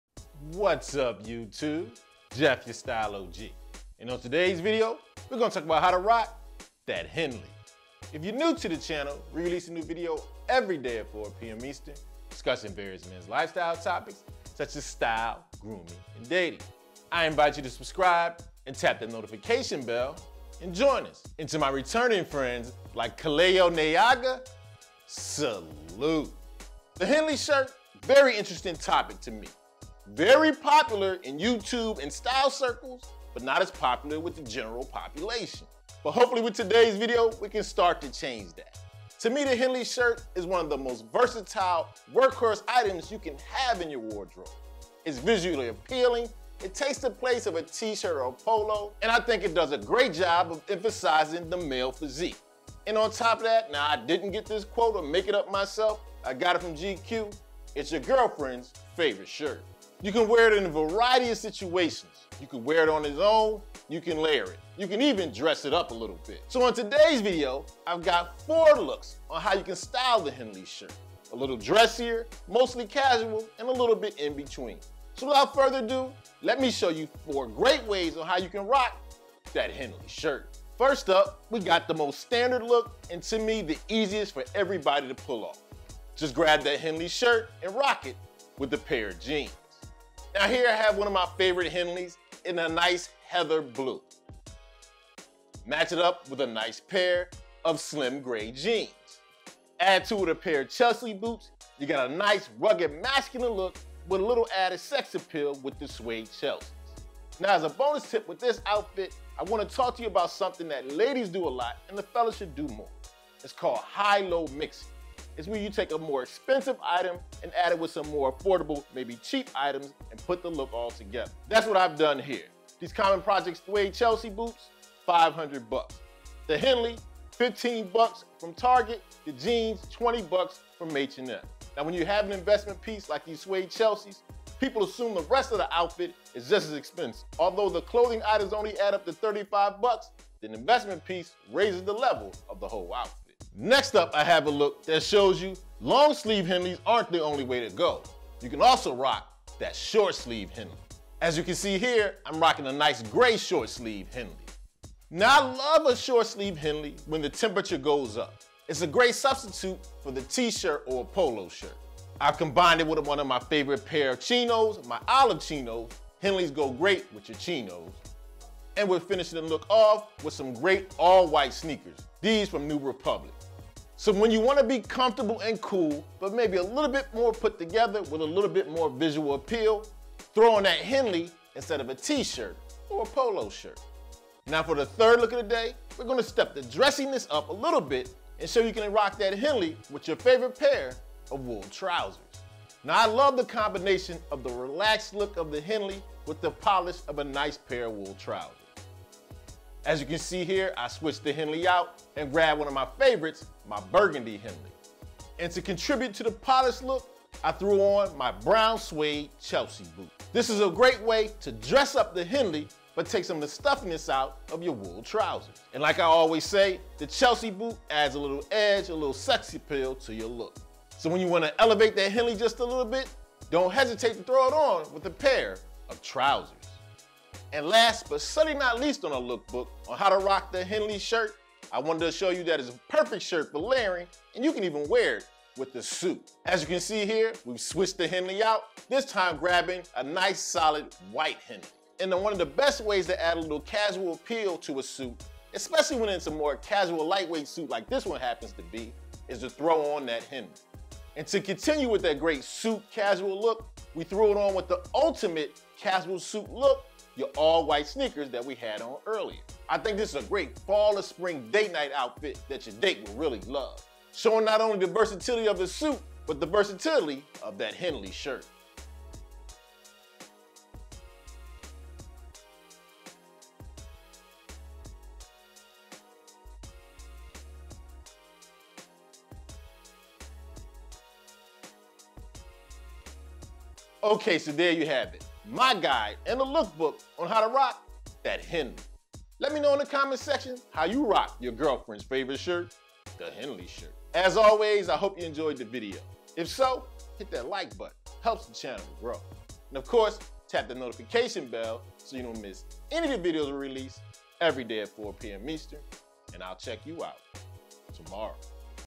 How To Wear A Henley Shirt
In this video, Jeff from The Style O.G. discusses how to wear a henley shirt. He shows a look book of four looks that show how versatile and stylish a henley can be.

Target Long Sleeve Henley
Target Olive Chinos

MY GEAR
CAMERA CANON 70D
LIGHTING KIT FANCIER 3 POINT LIGHTING
EXTERNAL MIC INSIGNIA MULTI DIRECTIONAL MIC